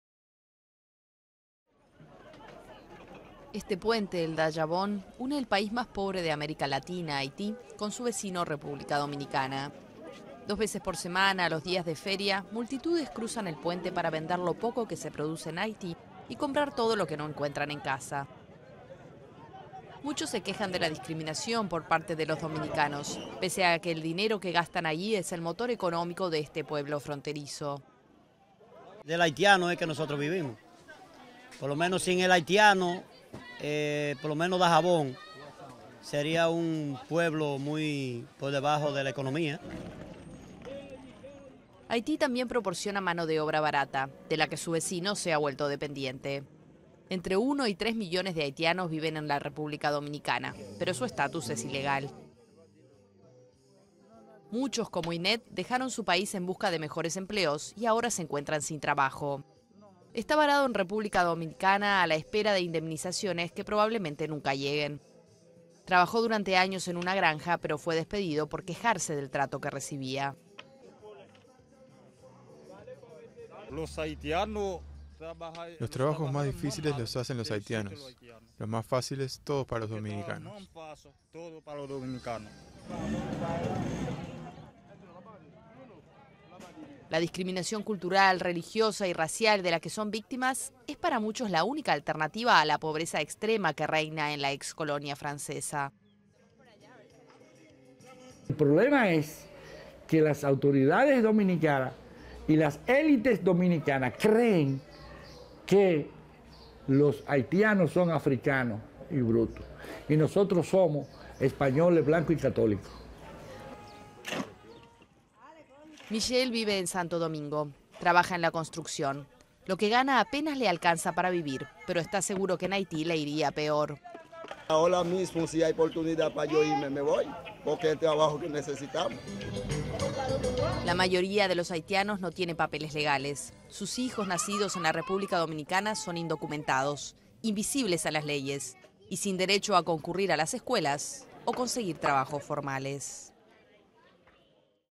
Haitianos discriminados
Haití es el país más pobre de América. La falta de oportunidades lleva a sus habitantes a cruzar la frontera en busca de trabajo en Republica Dominicana. Pero no siempre lo encuentran. La comunidad de haitianos afronta duras condiciones laborales, desempleo y discriminación.